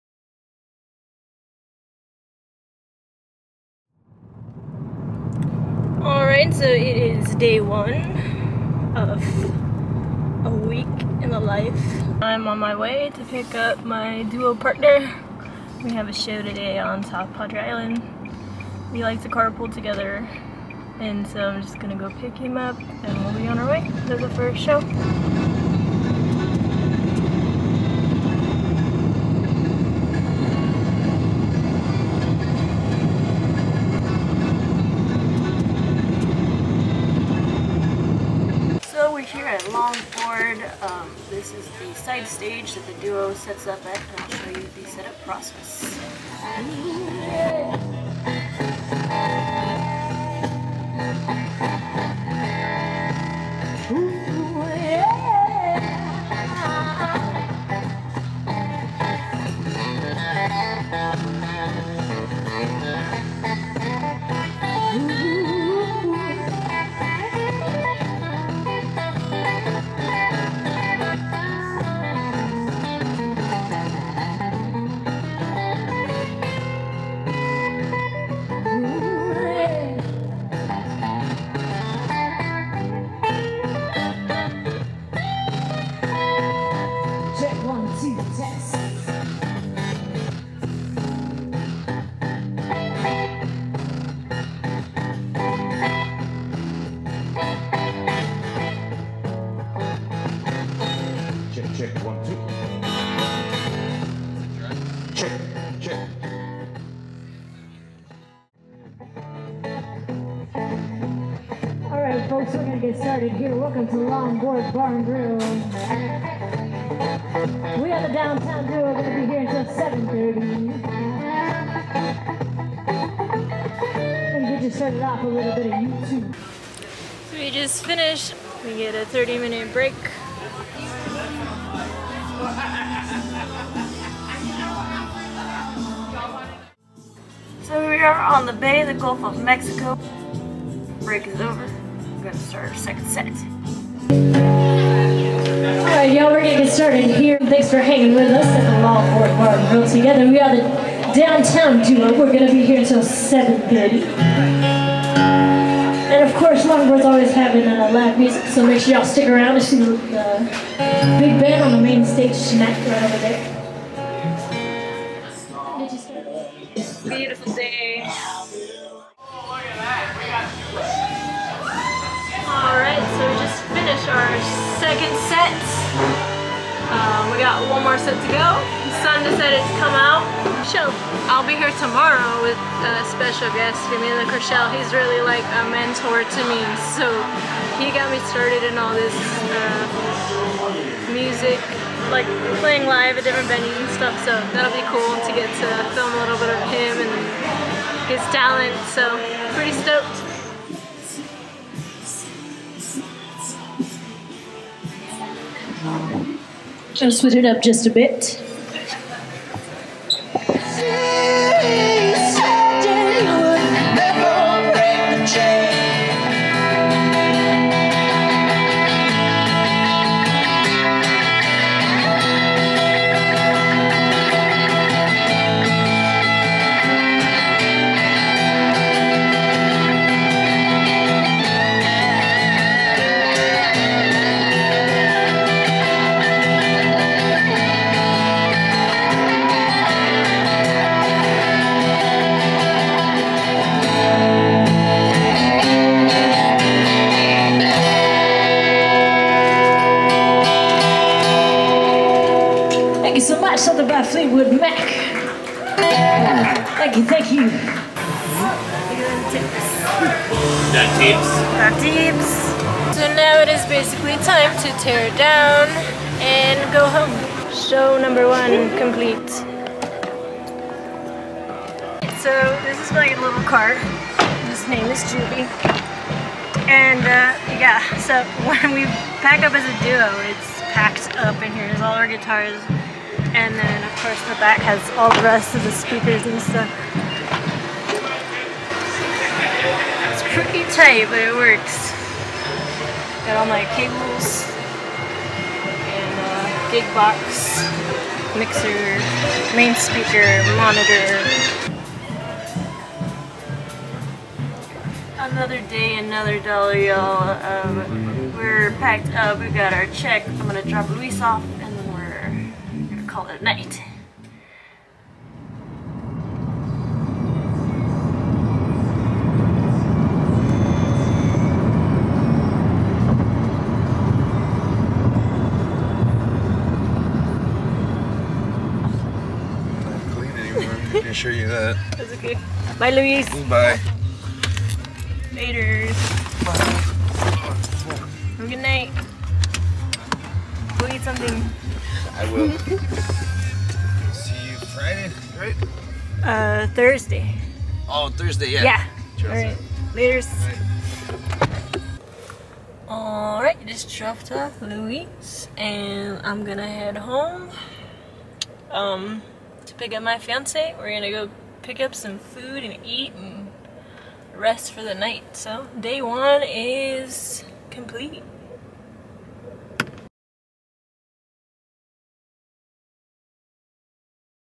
DAY 1- A Week in the Life of a Musician- IAMJEWELMARIE
Episode 1. Day one, Monday. April 2021

My first vlog episode of this series, follow me to my first show of the week performing with my duo partner Luis Foncerrada at Longboard Bar and Grill on South Padre Island Texas.

Episodes for this series to be released every other Monday! (up to day 7)